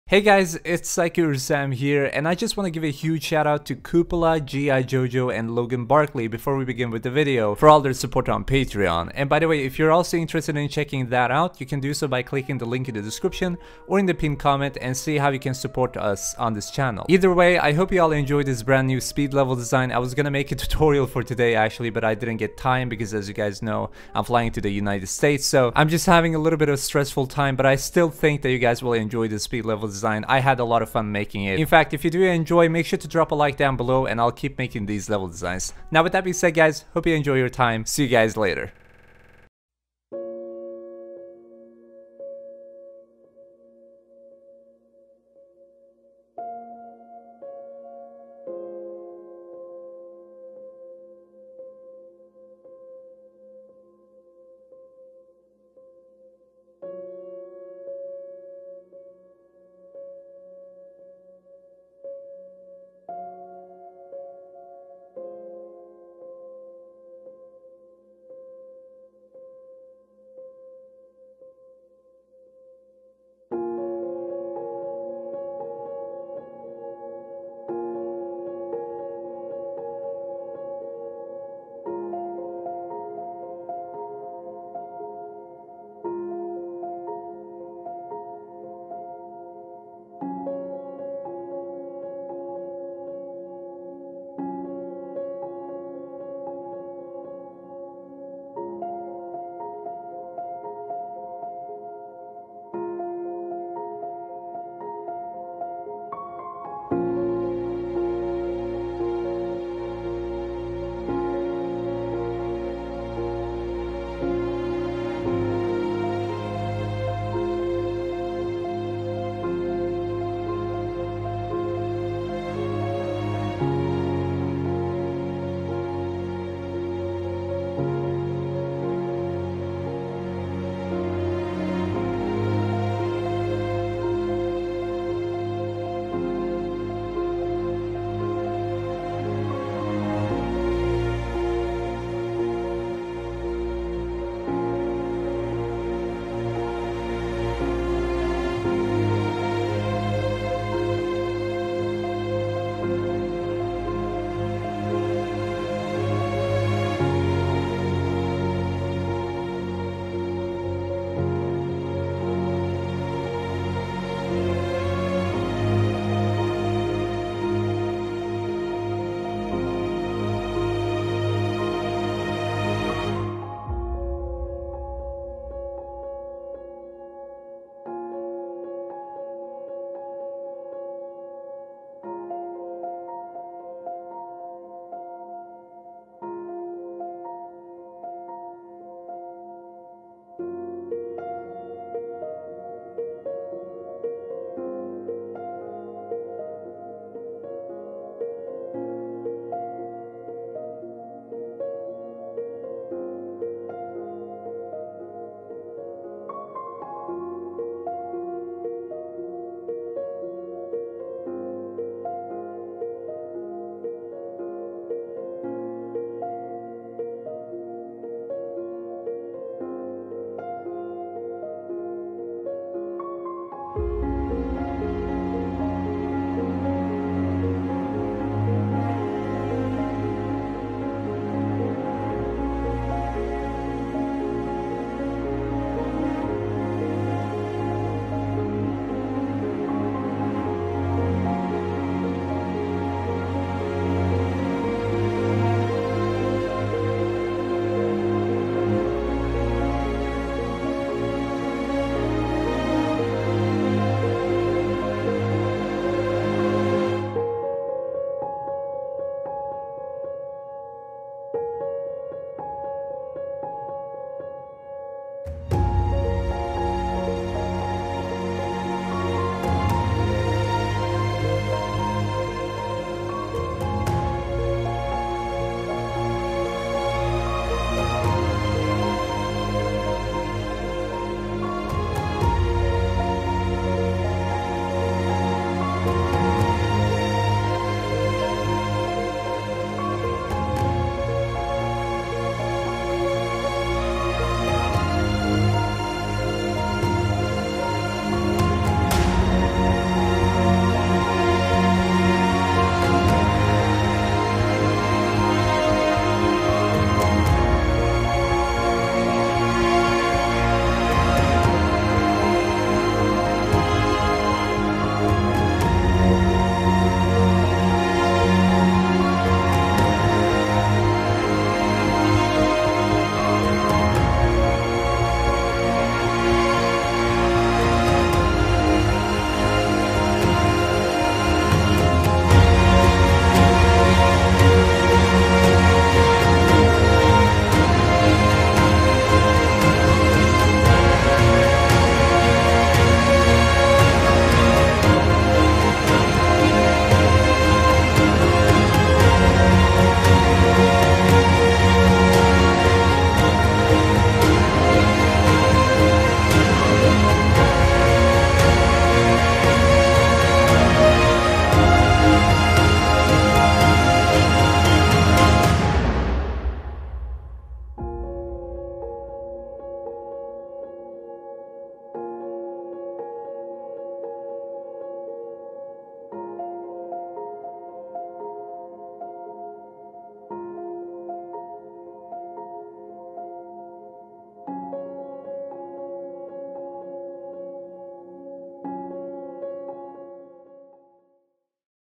Making a Level in Unity in 1 HOUR! | Speed Level Design
♥ VIDEO LIKE GOAL:
• 120 Likes!

Decided to make a level design today. Really felt like putting my imagination and creativity on the line. Hope you all enjoy!